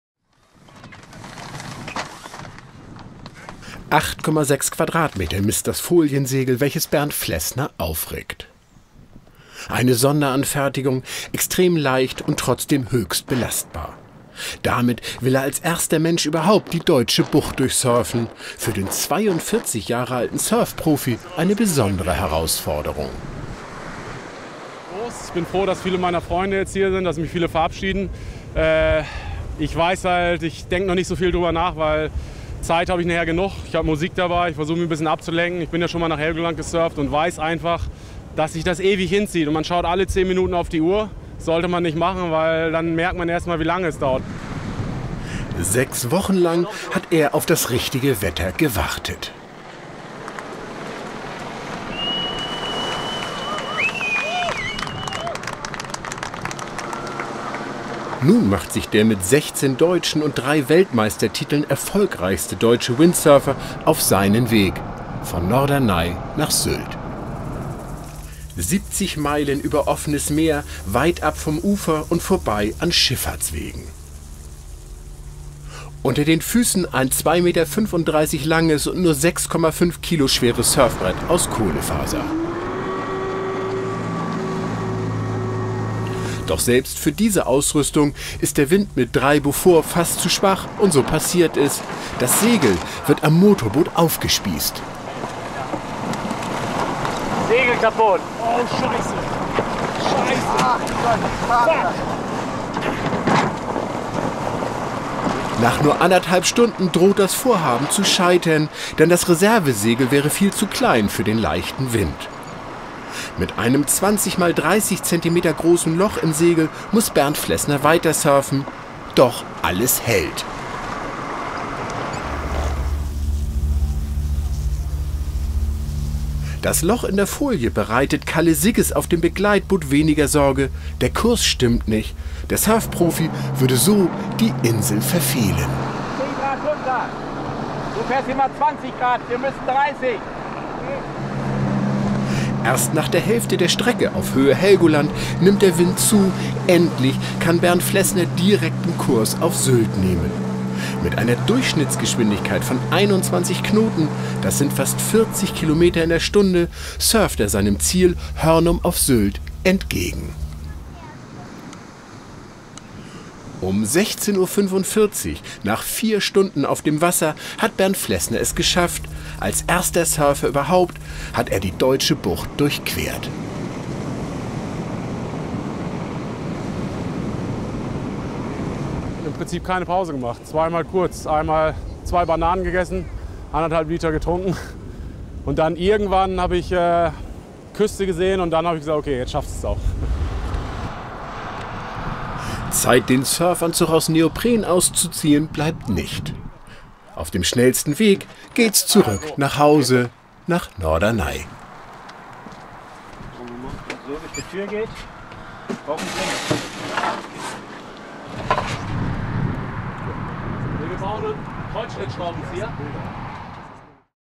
Bernd_Flessner_DeutscheBucht
German TV report: Bernd Flessner crossed the German Bight.